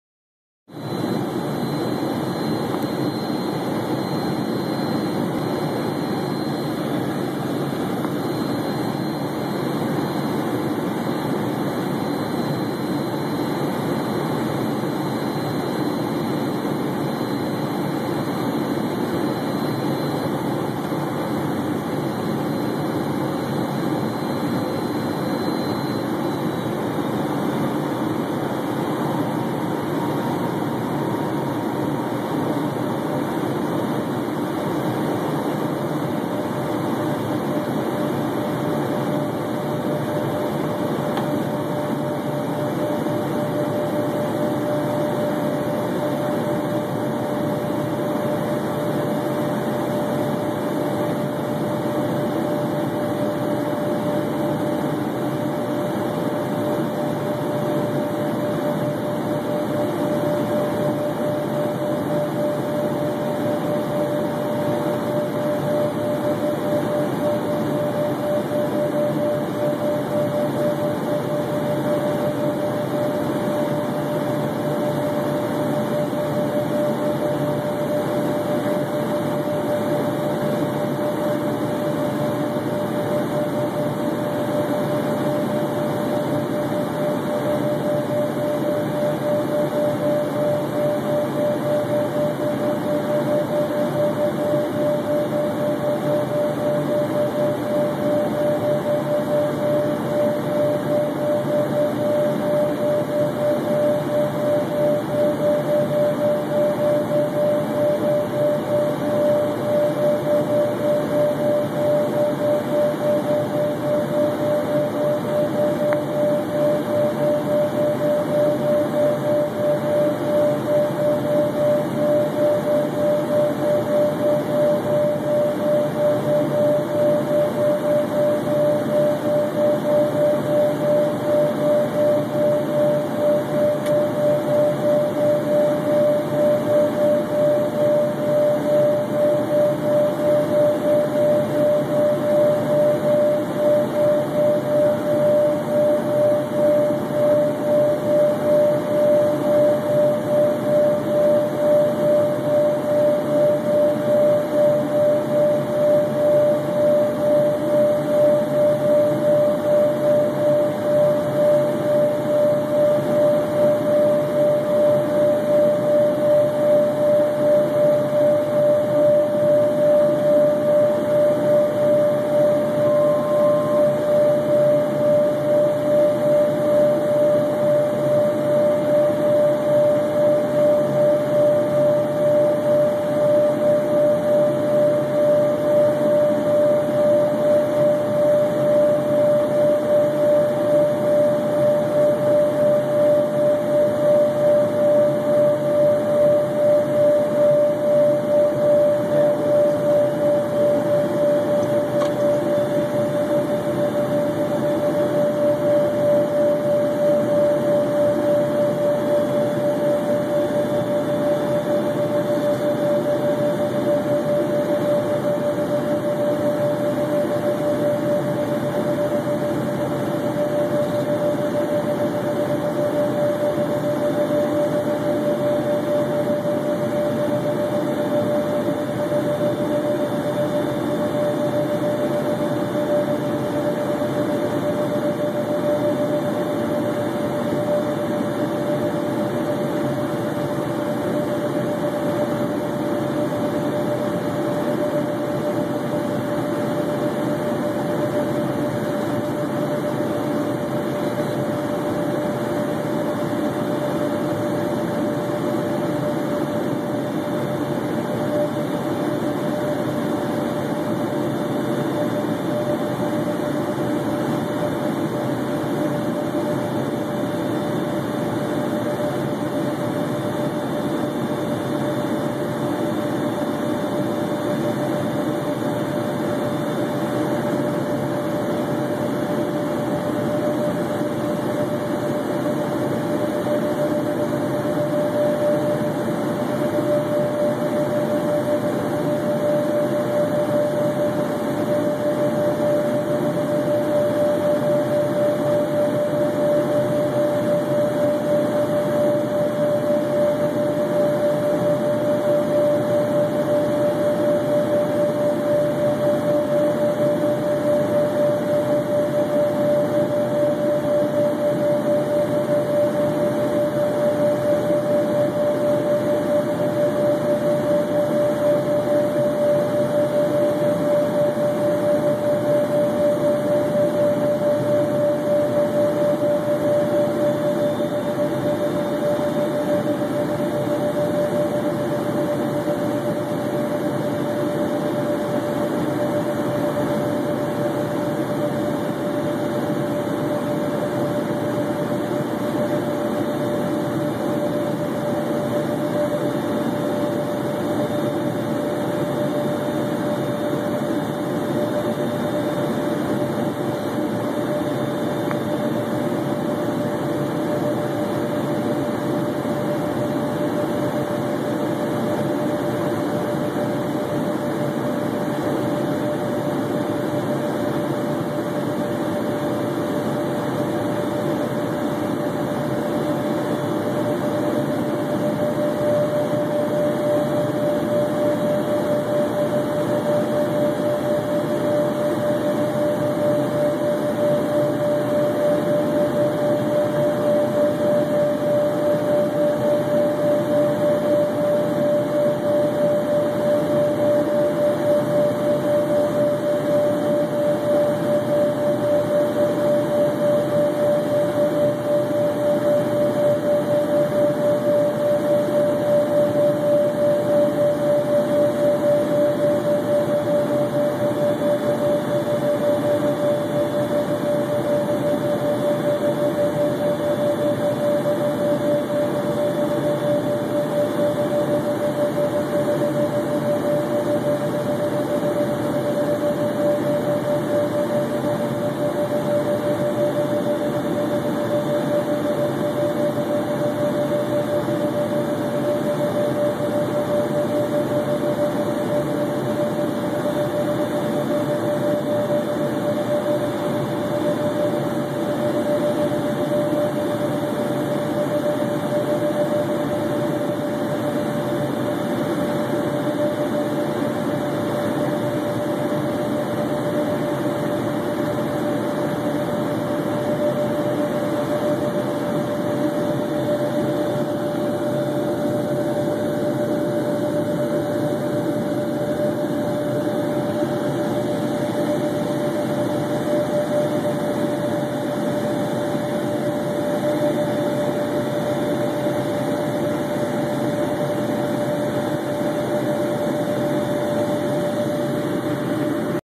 Phenom 100 De-Ice Boots In-Flight
Purdue's Phenom 100 descending through the clouds with view of de-ice boots cycling.